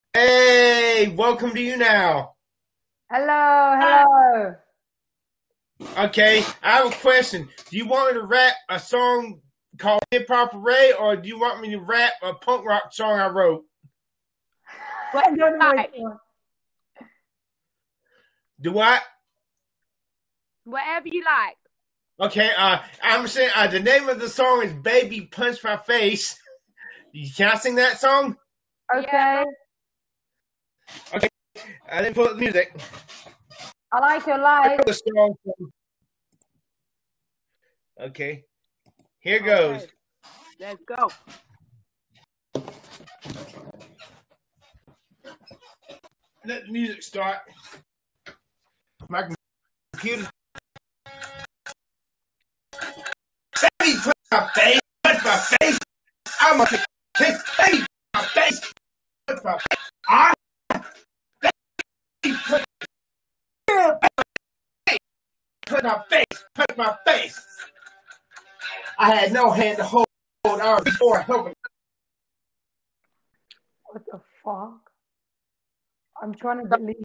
🤮🤯🤯 Tetyana TANIA QUITE SHOCKED AND SHAKEN by my singing BABY PUNCH MY FACE 😲😲😲
I mean, as mega-successful and rich as she IS, MUST SHE TEAR ME DOWN to feel good about herself

SHE has 38.6 K followers on Instagram...

Gets over 1,000,000 every time she broadcasts on younow.com...

ME, Wild Man Chris?

474 followers on Instagram
Have only 65 followers on my Twitter